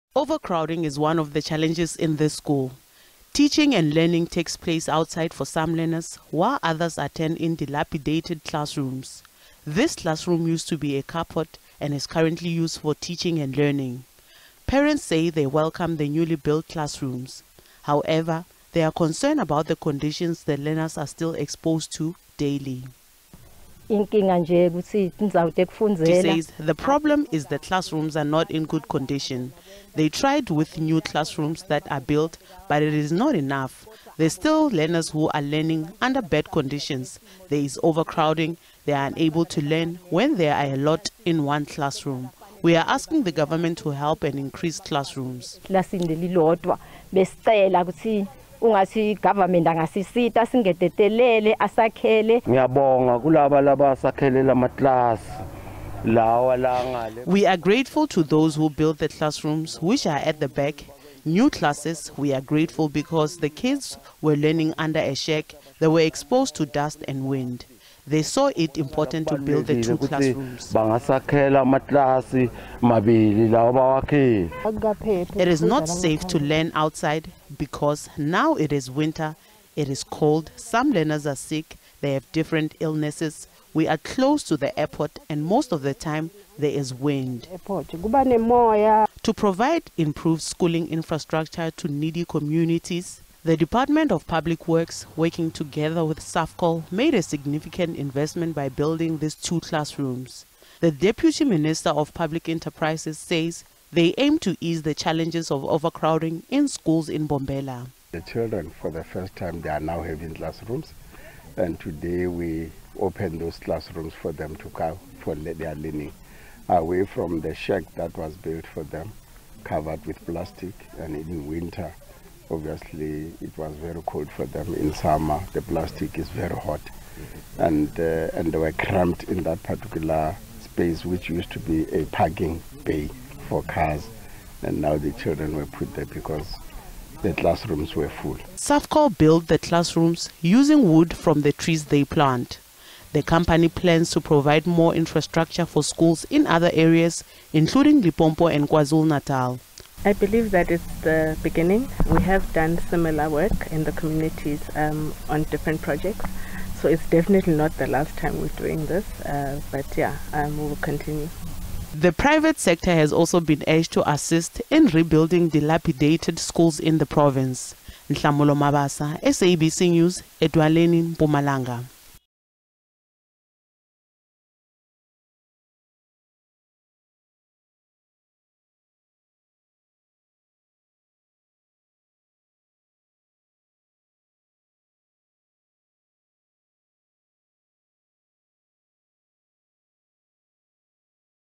Parents urge government to build more classrooms at a Mpumalanga school
Parents of learners at Edwaleni Primary School in Kabokweni in Mpumalanga are calling for the government to build more classrooms at the school. Learning and teaching take place in overcrowded and dilapidated buildings at the school.